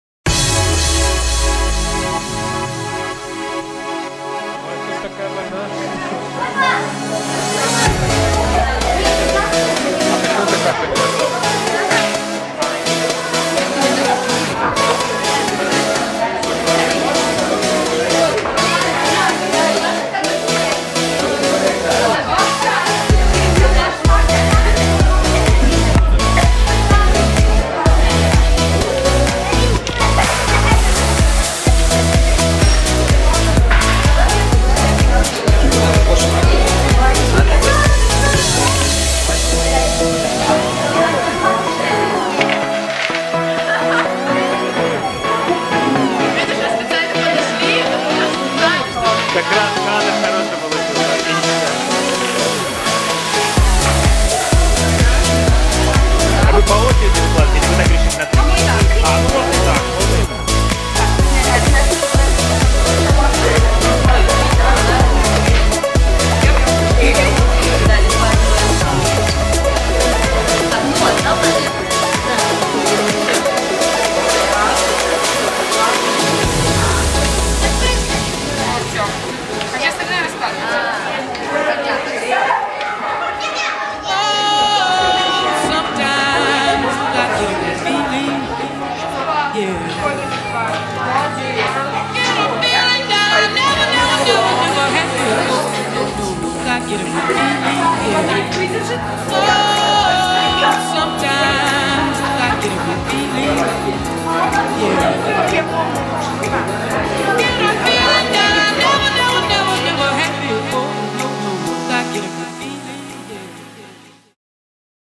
Барановичи - игротека 22.04.2012
ИГРОТЕКА проходила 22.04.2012 в Центральной библиотеке им. В. Тавлая по адресу - ул. Ленина, 51